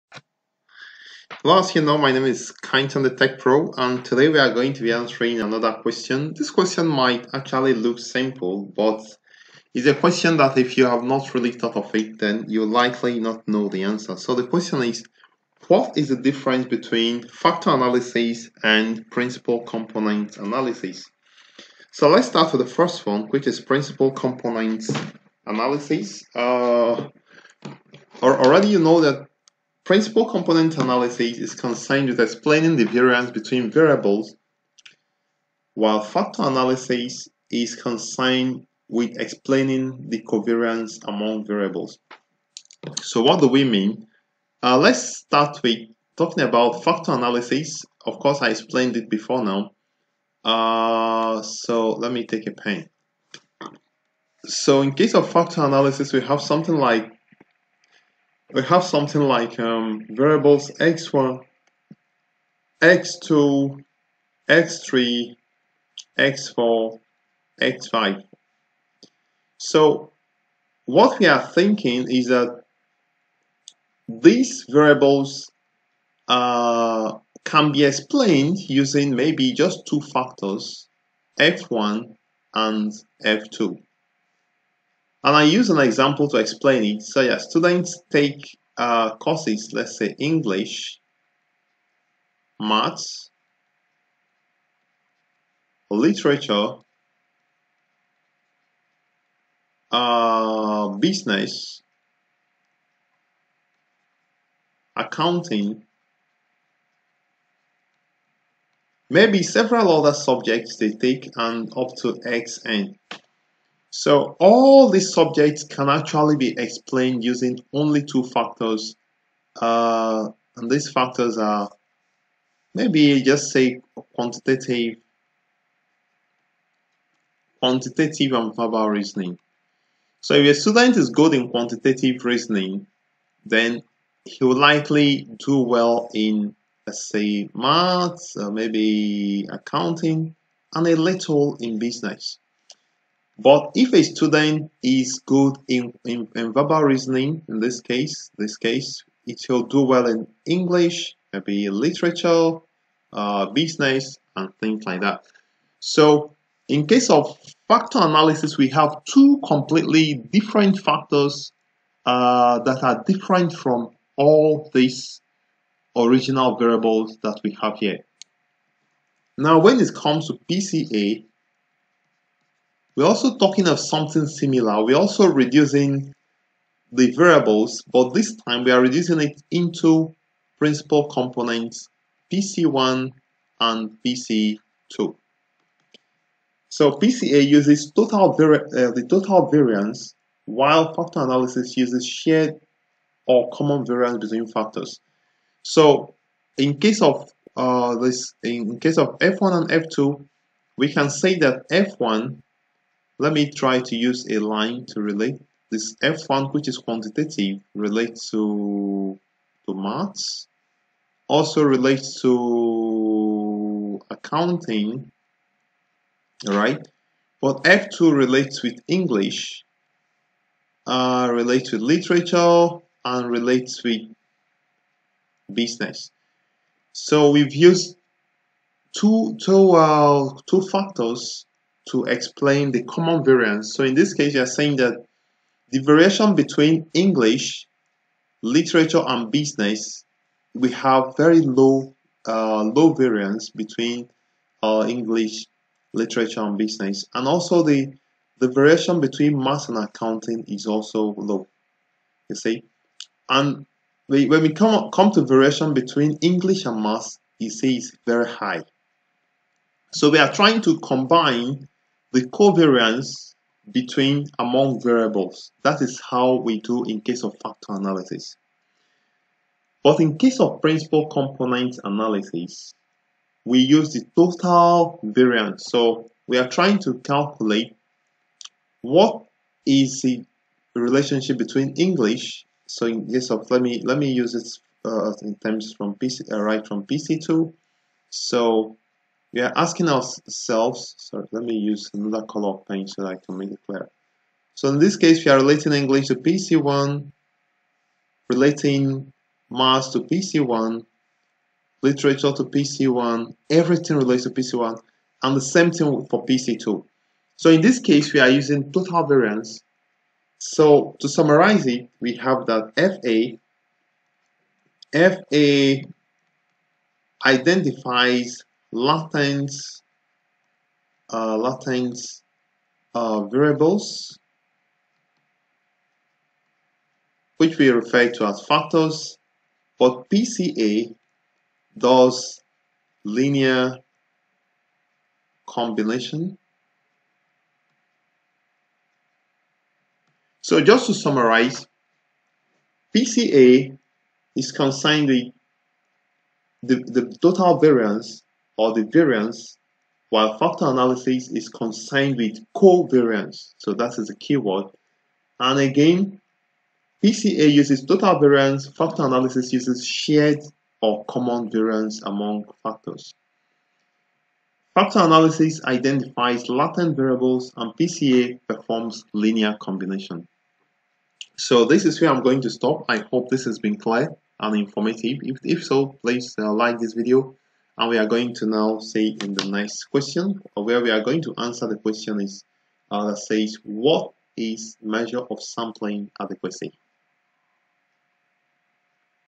Question 16 - Differentiate Between Factor Analysis and Principal Components Analysis
Data Science Questions and Answers